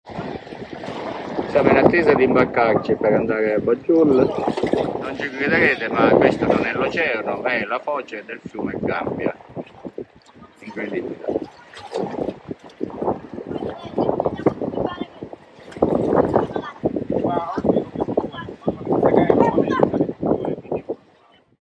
Missione Gambia Il fiume Gambia
Missione Gambia: le immagini impressionanti della foce del fiume Gambia, che da il nome all'intera Nazione